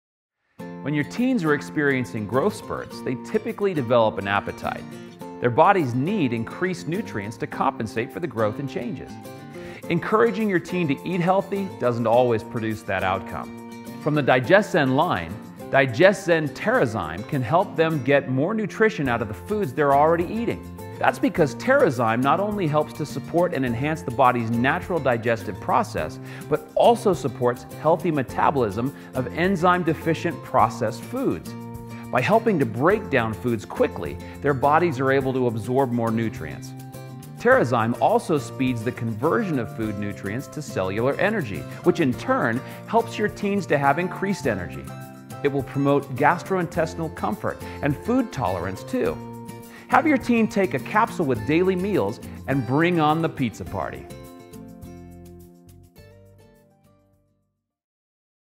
Terrazyme & Teenagers
When your teens are experiencing growth spurts, they typically develop an appetite. Their bodies need increased nutrients to compensate for the growth and changes. Encouraging your teen to eat healthy doesn’t always produce that outcome. From the DigestZen line DigestZen TerraZyme can help them get more nutrition out of the foods they’re already eating.
Featured product: TerraZyme